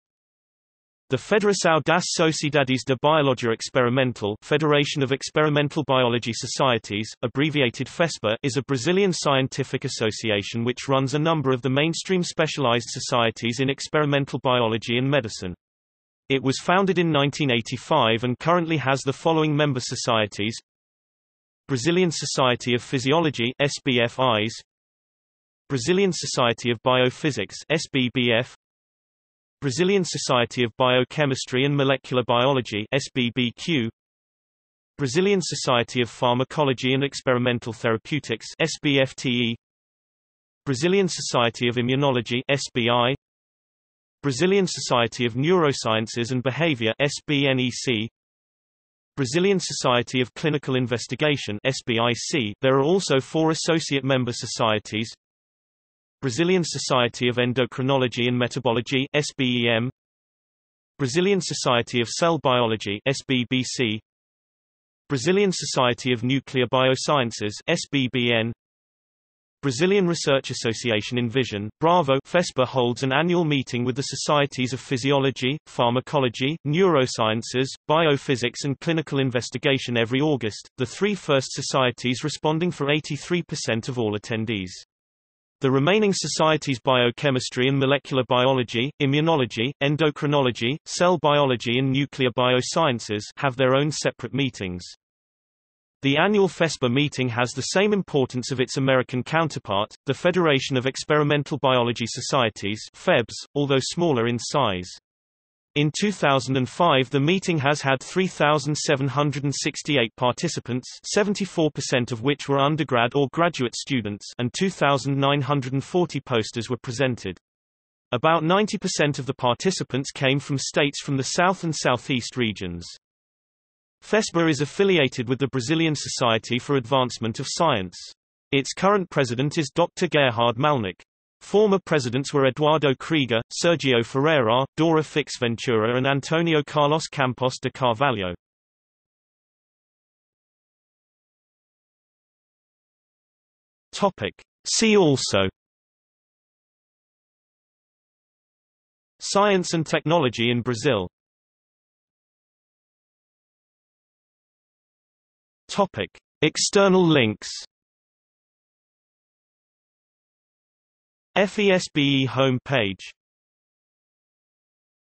Federação das Sociedades de Biologia Experimental | Wikipedia audio article
00:02:20 See also
00:02:32 External links

- Socrates

SUMMARY
The Federação das Sociedades de Biologia Experimental (Federation of Experimental Biology Societies, abbreviated FeSBE) is a Brazilian scientific association which runs a number of the mainstream specialized societies in experimental biology and medicine. It was founded in 1985 and currently has the following member societies:

Brazilian Society of Clinical Investigation (SBIC)There are also 4 associate member societies:

Brazilian Society of Endocrinology and Metabology (SBEM)
Brazilian Society of Cell Biology (SBBC)
Brazilian Society of Nuclear Biosciences (SBBN)
Brazilian Research Association in Vision (BRAVO)FeSBE holds an annual meeting with the societies of physiology, pharmacology, neurosciences, biophysics and clinical investigation every August, the three first societies responding for 83% of all attendees. The remaining societies biochemistry and molecular biology, immunology, endocrinology, cell biology and nuclear biosciences) have their own separate meetings.
The Annual FeSBE Meeting has the same importance of its American counterpart, the Federation of Experimental Biology Societies (FEBS), although smaller in size. In 2005 the Meeting has had 3,768 participants (74% of which were undergrad or graduate students) and 2,940 posters were presented. About 90% of the participants came from states from the South and Southeast regions.